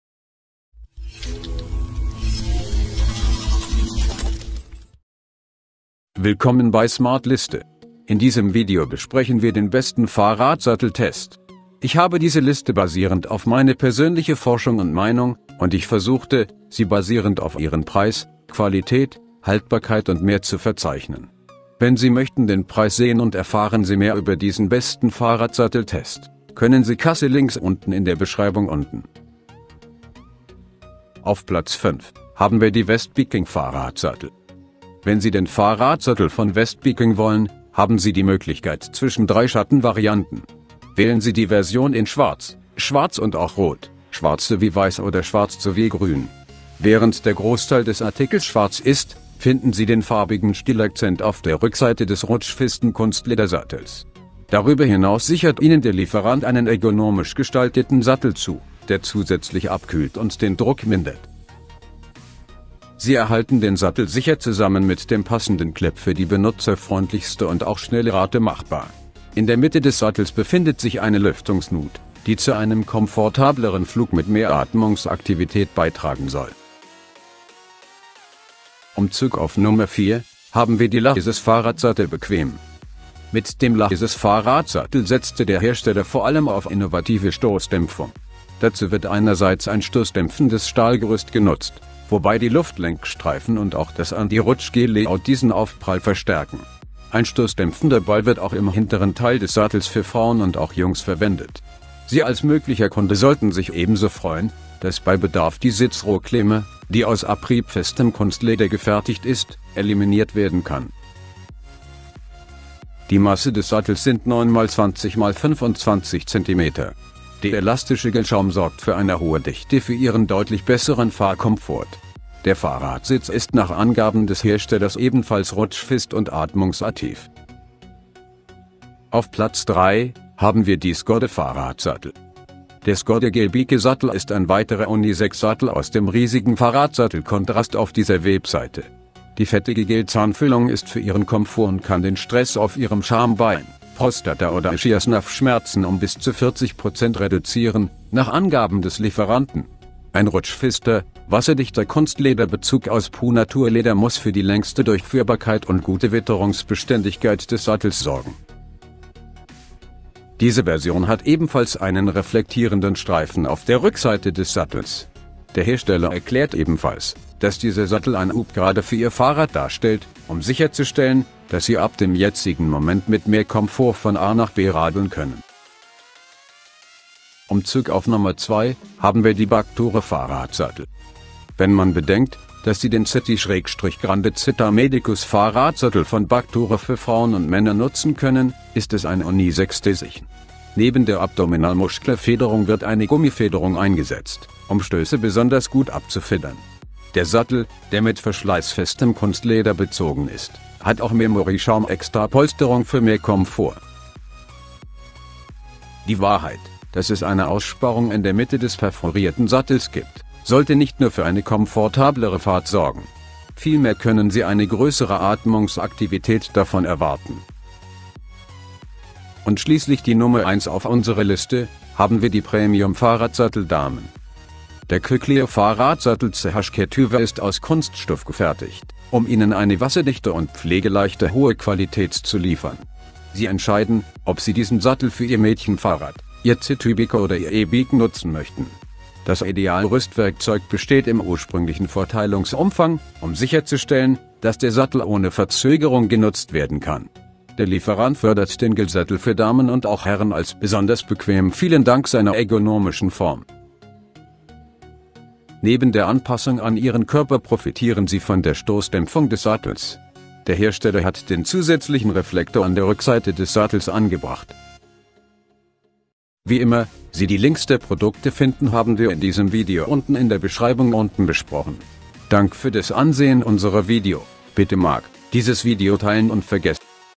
Die Besten Fahrradsattel Test - (Top 5)
Hier ist die Liste von Die 5 Besten Fahrradsattel Test

1. Premium Fahrradsattel Damen

2. BACKTURE Fahrradsattel

3. SGODDE Fahrradsattel

4. Lachesis Fahrradsattel Bequem

5. West Biking Fahrradsattel